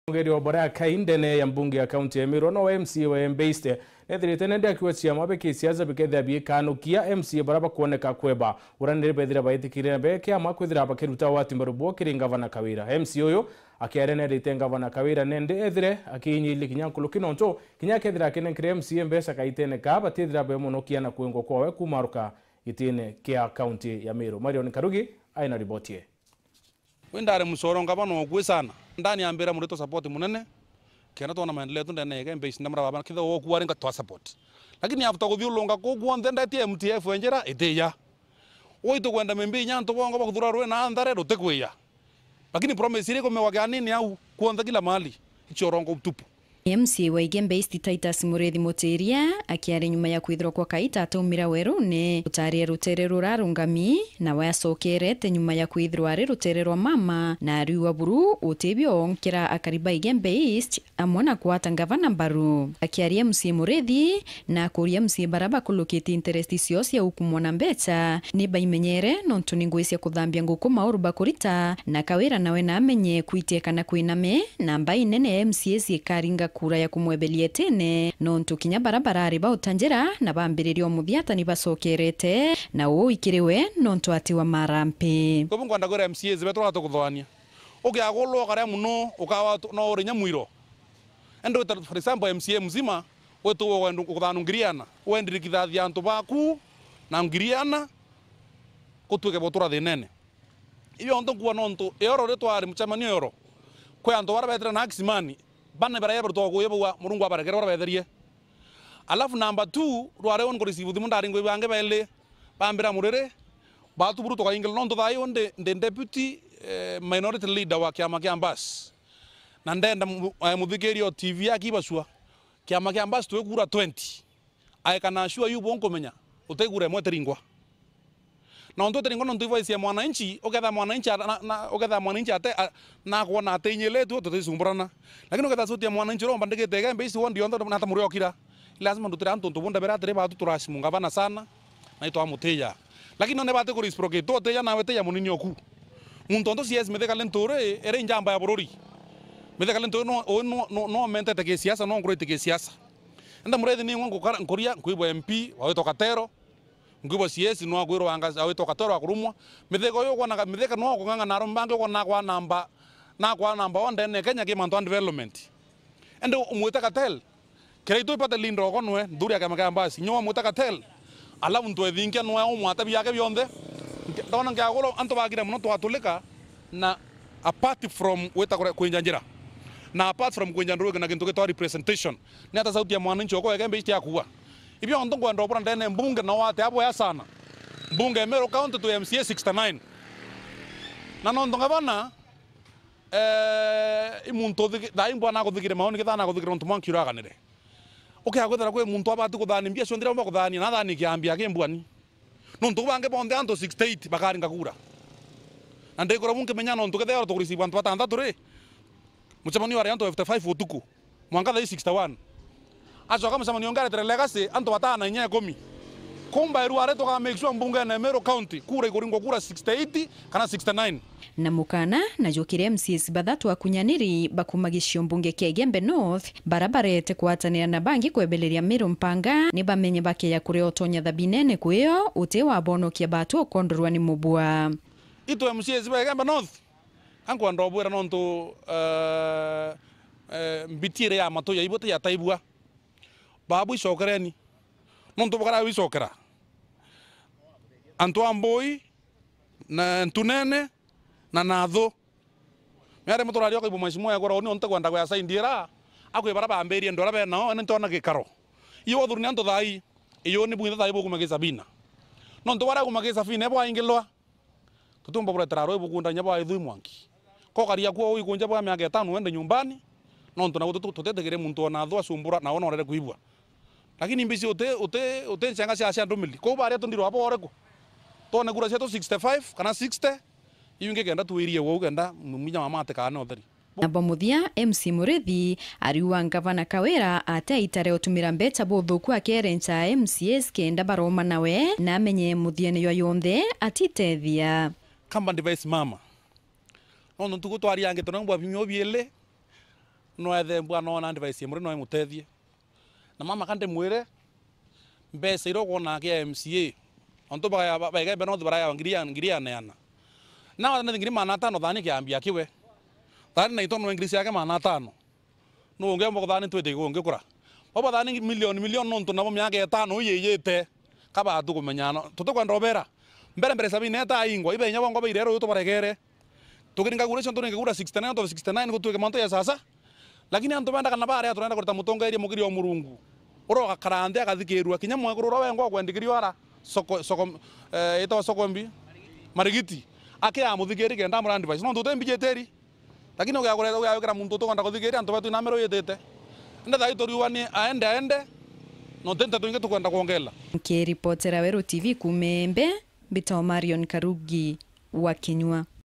KITHANJU KIA BIAMA
MCA BAREA BATIKWATHIKIRA BIAMA BIAO KUJUKIRUA MATAGARIA II BIAMA
IKWITHIRITWE NA BAMWE BA MCA BAGUCEMANIA NA MAMA GIUTATI NA WATHO
MCA MURII: KAWIRA UGATINDA KWINYANGIA MBECA UKAANA KAGITA GAKA